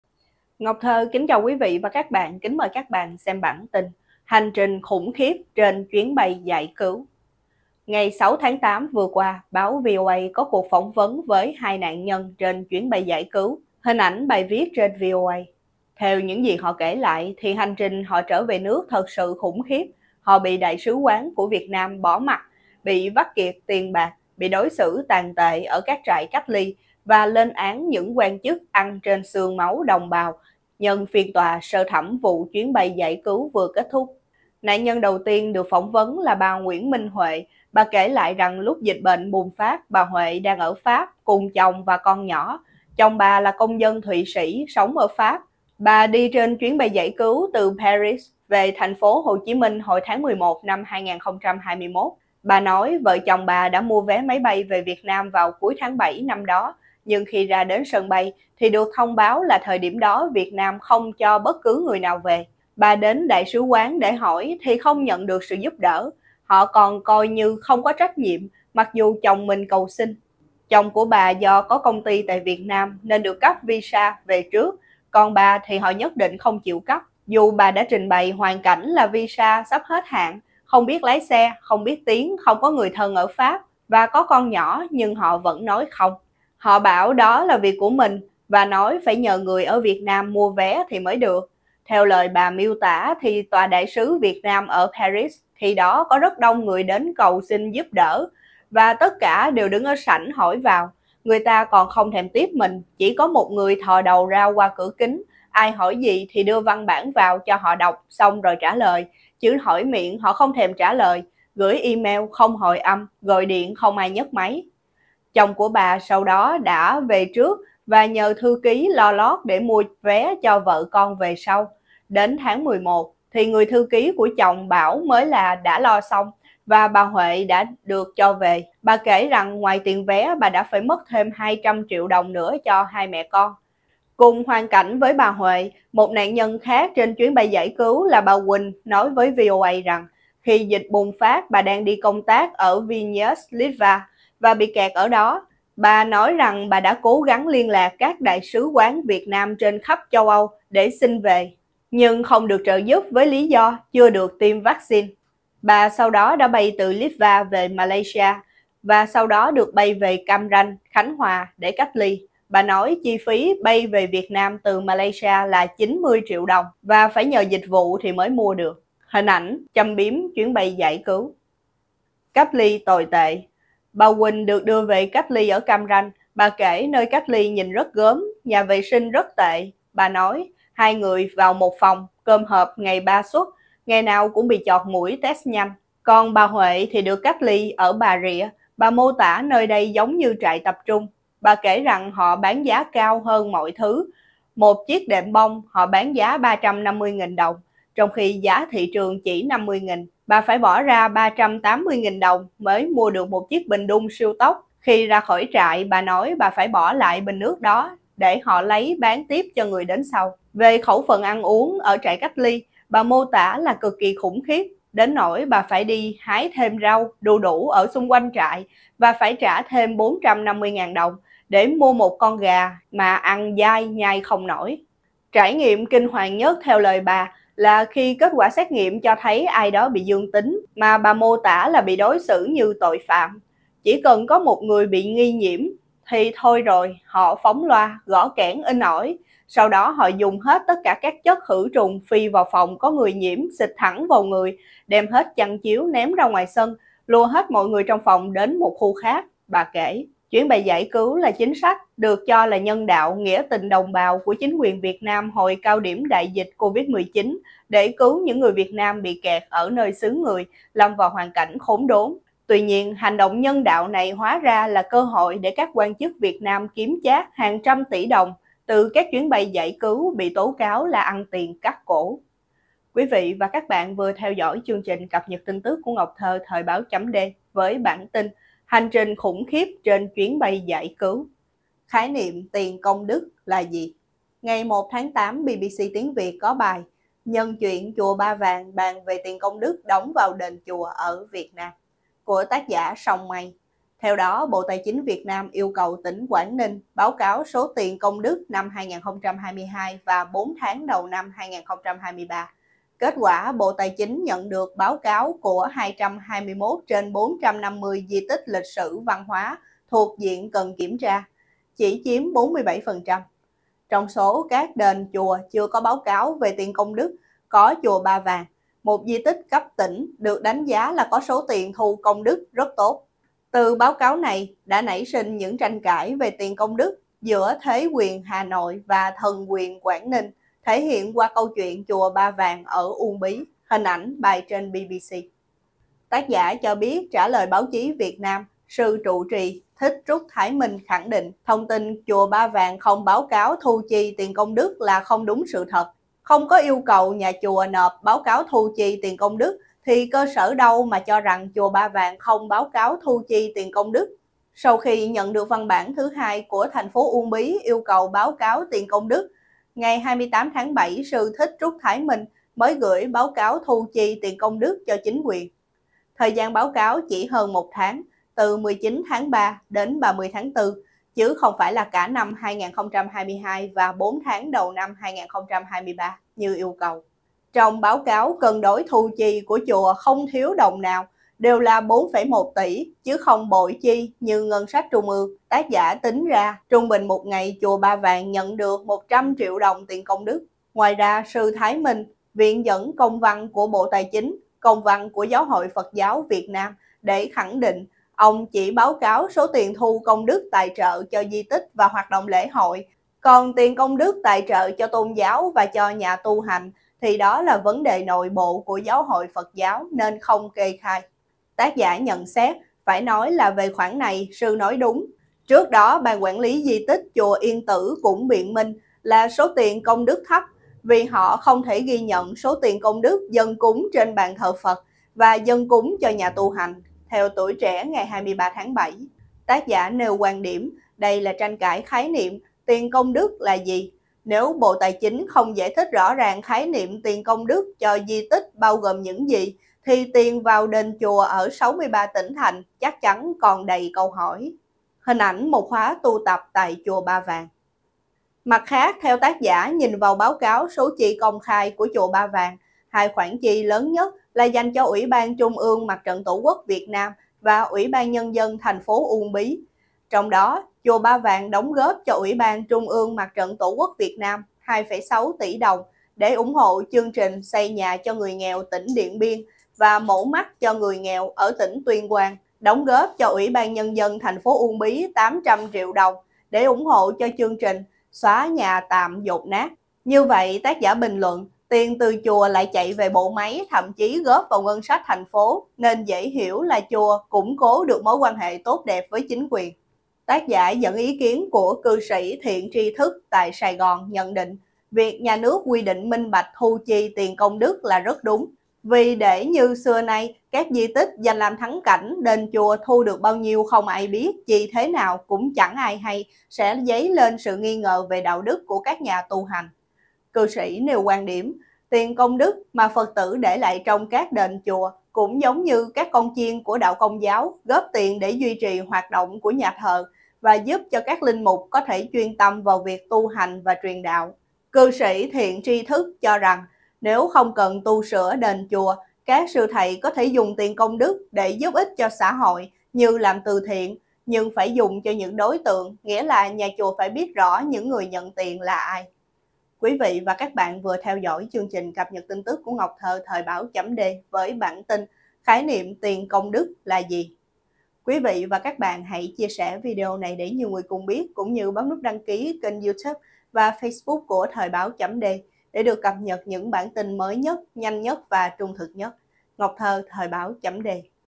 4000 tỷ của dân quan, đảng nuốt sạch – dân bị vặt trụi lông gà!
‘Chuyến bay giải cứu’ là chính sách được cho là ‘nhân đạo’, ‘nghĩa tình đồng bào’ của chính quyền Việt Nam hồi cao điểm đại dịch COVID-19 để cứu những người Việt bị kẹt lại nơi xứ người, lâm vào cảnh khốn đốn.
Bạn cài Ultrasurfing để vượt tường hoặc xem qua link vượt tường . Thoibao.de từ Berlin, CHLB Đức.